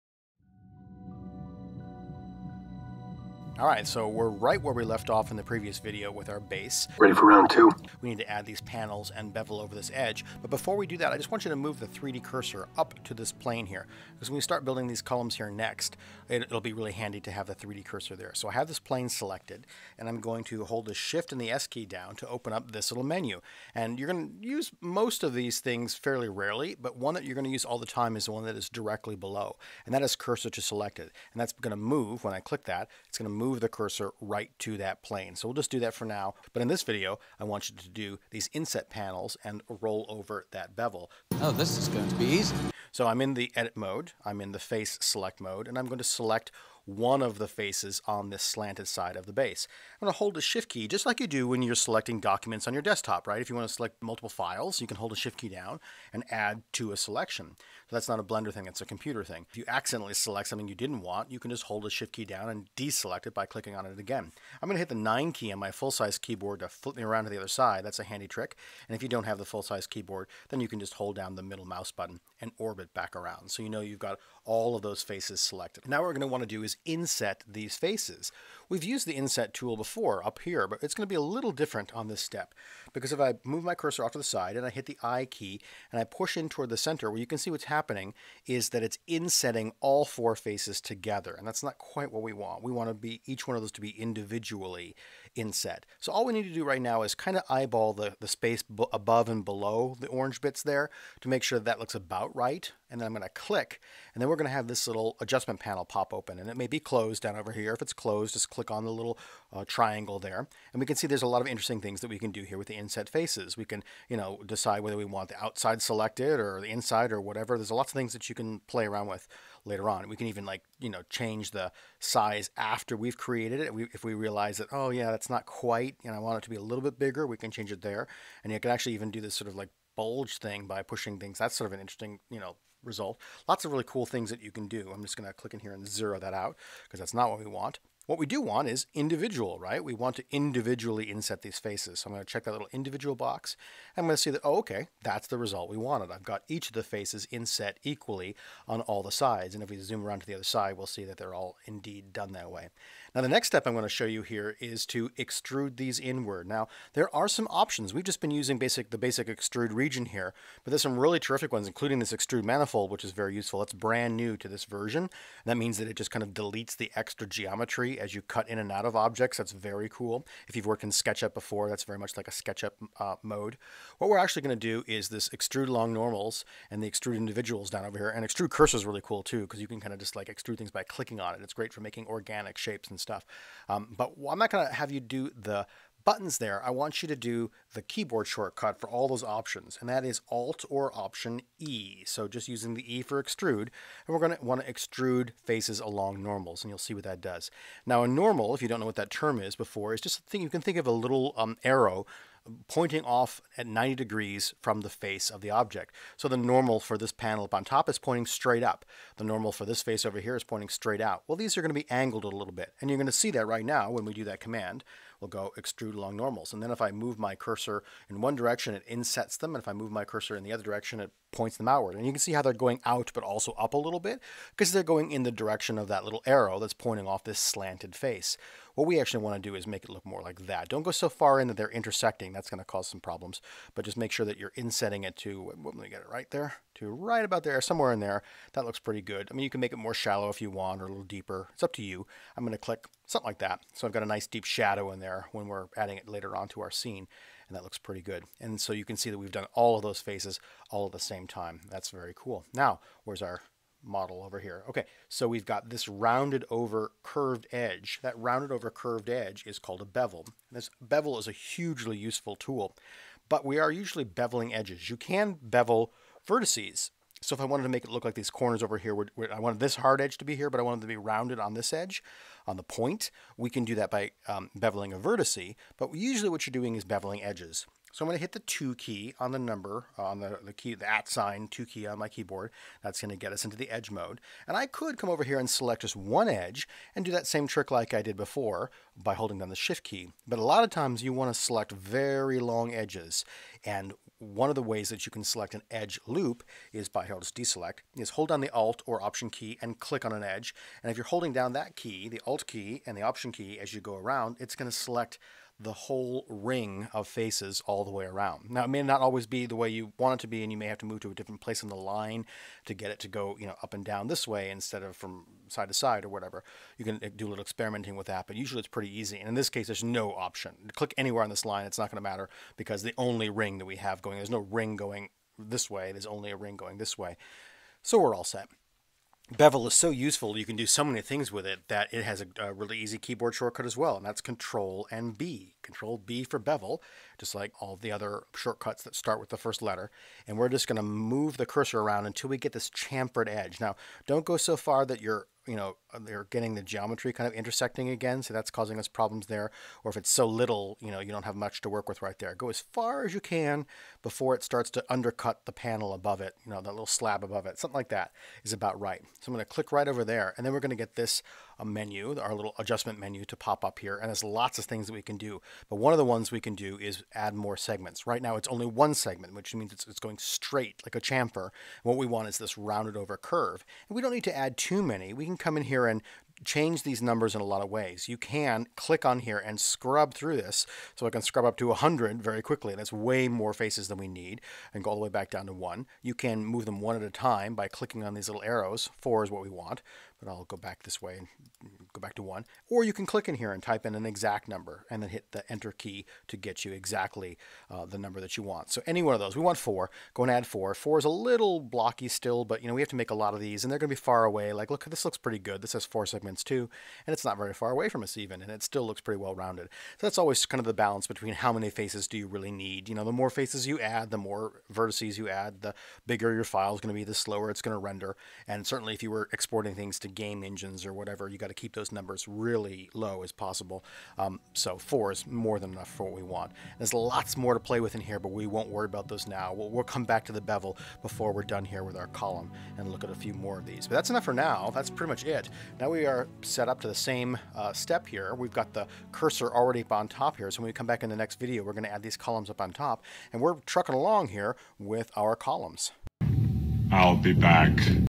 Blender Beginner Dungeon Part 3
This introduction Blender tutorial takes beginners through the process of building a dungeon scene while demonstrating Blender’s core concepts. This third video shows demonstrates the inset and bevel tool to make the column base more interesting.